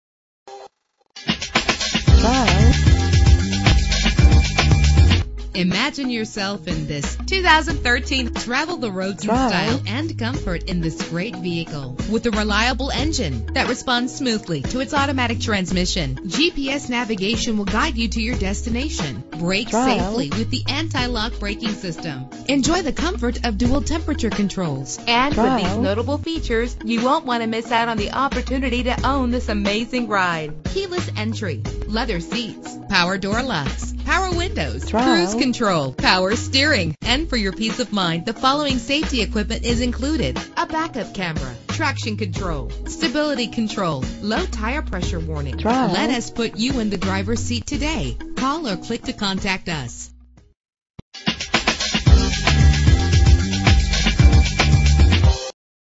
2013 Cadillac ATS - Kokomo IN
Year: 2013
Make: Cadillac
Model: ATS
Trim: 2.5L Luxury Sedan 4D
Engine: 4 cylinder 2.5 Liter
Transmission:
Color: Blue
Mileage: 22
Address:
1220 East Boulevard